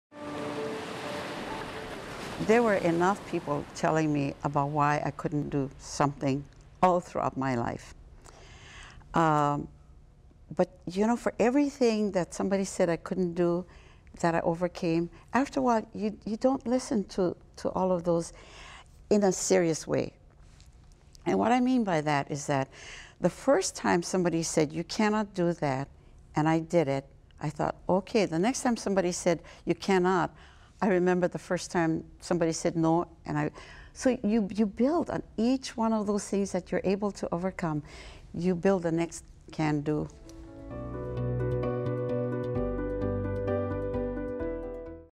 Overcoming Naysayers | aloha&grit
Longtime Native Hawaiian business owner Momi Cazimero shares how she overcame naysayers.